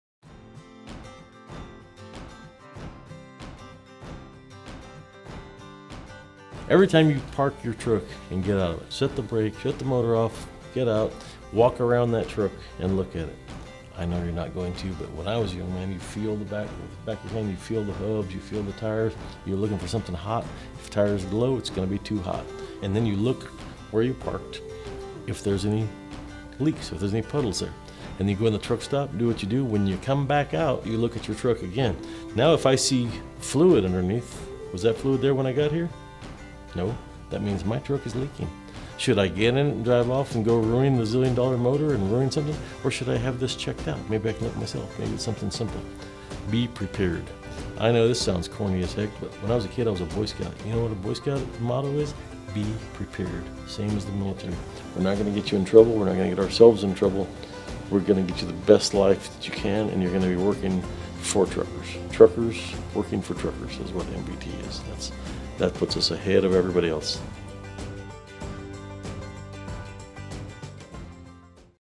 Trucker Talk Tuesday | President & CEO Royal Jones Informs Us About G.O.A.L
Our first episode of Trucker Talk Tuesday! President & CEO, Royal Jones gives a piece of advice to truck drivers.